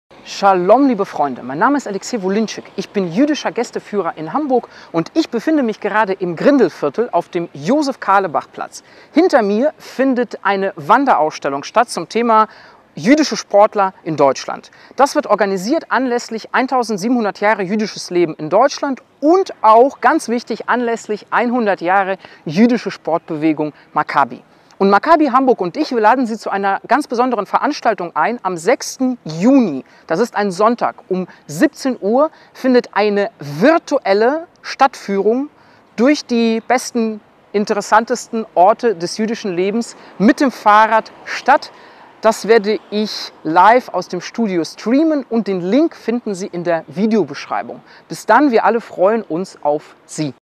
TRAILER: Virtuelle Führung “Mit dem Fahrrad durchs jüdische Hamburg”
In diesem Jahr feiern wir nicht nur 1700 Jahre jüdisches Leben in Deutschland, sondern auch ein anderes schönes Jubiläum: 100 Jahre jüdische Sportbewegung Makkabi.

Zu diesem Anlass lädt Sie Makkabi-Hamburg am 6 Juni um 17:00 zu einer besonderen Veranstaltung ein:
der virtuellen Führung "Mit dem Fahrrad durchs jüdische Hamburg" mit dem jüdischen Gästeführer Alexei Volinchik.

Die Führung wird live aus dem Studio Alexander Klütschnik KOD
Gesendet und vereint den Sportgedanken mit der jüdischen Geschichte unserer Stadt.